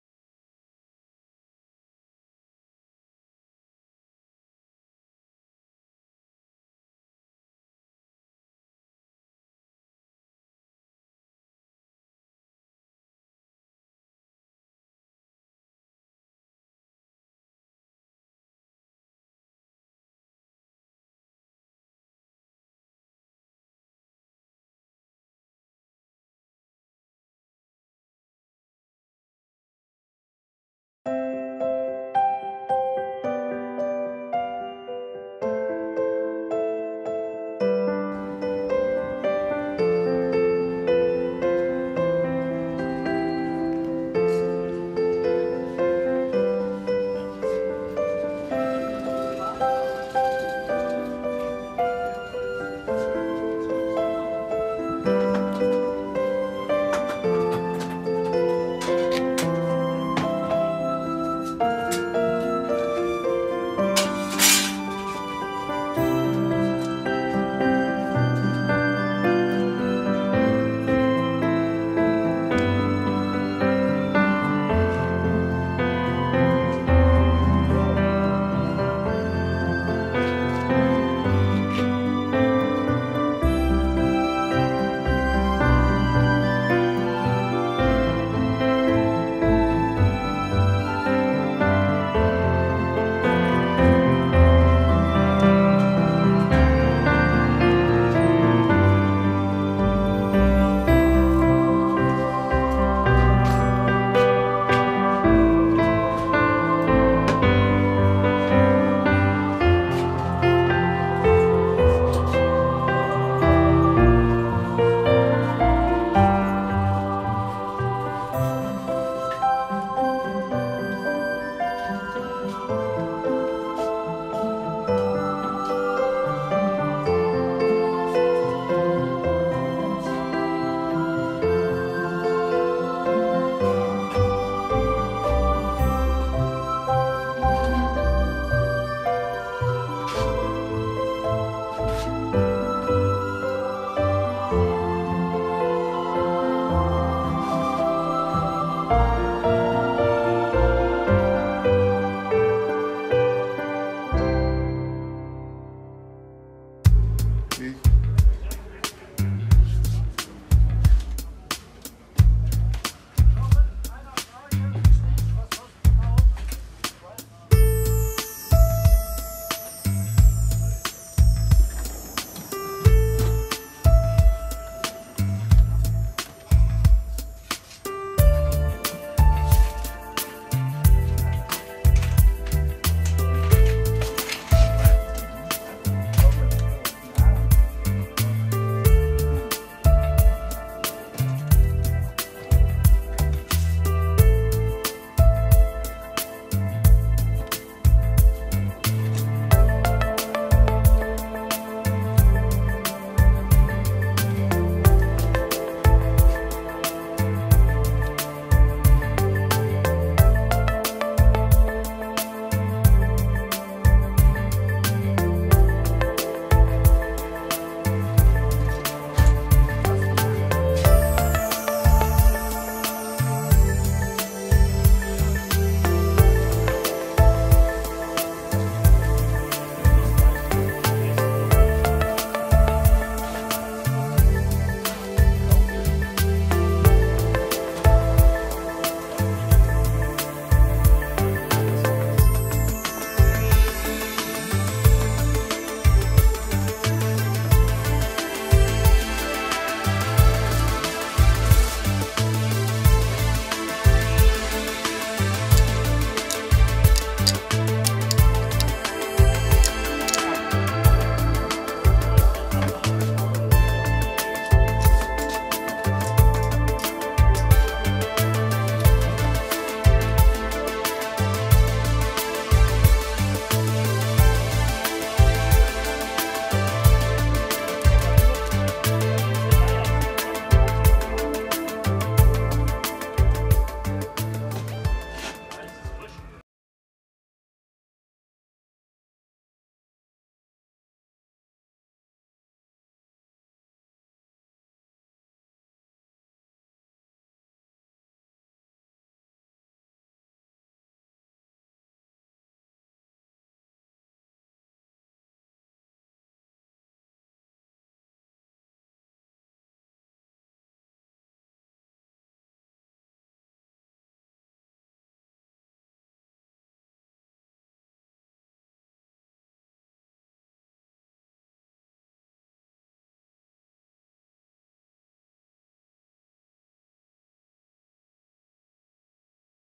Heinkel Shop | Hinter den Kulissen
Der mobile Heinkel Shop versorgt Euch auf Treffen und Veranstaltungen mit den notwendigen Heinkel Ersatzteilen.
Für Intessierte Mitglieder des Clubs gibt es nun einen Blick hinter die Kulissen. Viel Spaß